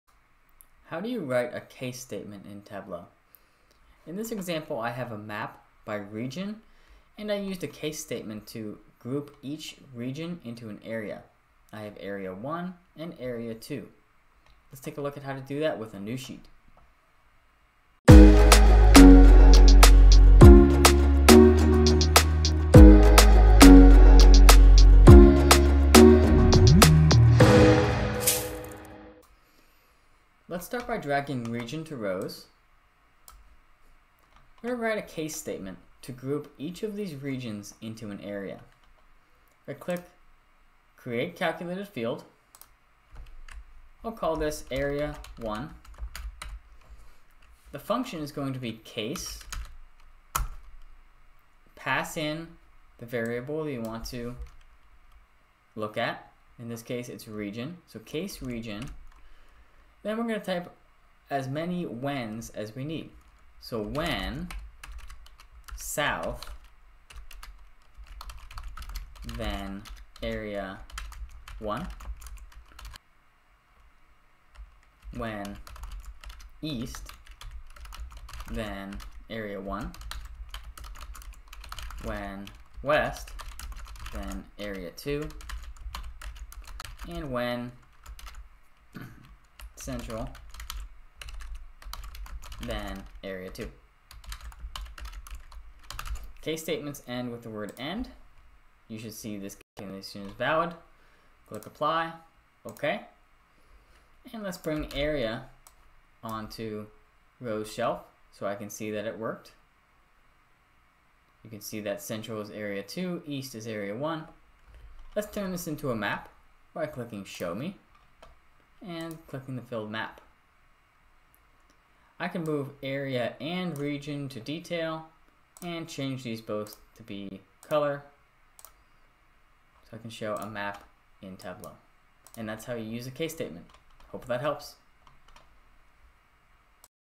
How to Write a Case Statement in Tableau
In this example I have a map by region. I used a case statement to group each region into an area. I have Area 1 and area 2.

Let's start by dragging region to rows I'm gonna write a case statement to group each of these regions into an area. If I click create calculated field I'll call this area one the function is going to be This. case, in the variable you want to look at in this case it's region so case region then we're going to type as many when's as we need so when South then area one. When East then area one. West then area two and when Central then area two.

Case statements end with the word end.

Let's bring area onto row shelf so I can see that it worked. You can see that Central is area two East is area one . Let's turn this into a map by clicking show me and clicking the field map I can move area and region to detail and change these both to be color. I can show a map in Tableau.